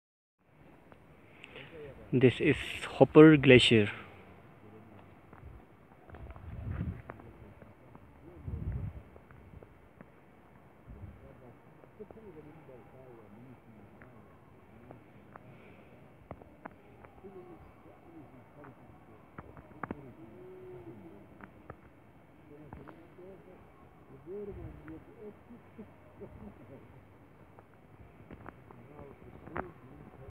Hoper glacier Nagar Valley Gilgit Baltistan
Hoper Glacier one hour Drive from Hunza located in Nagar Valley Pakistan.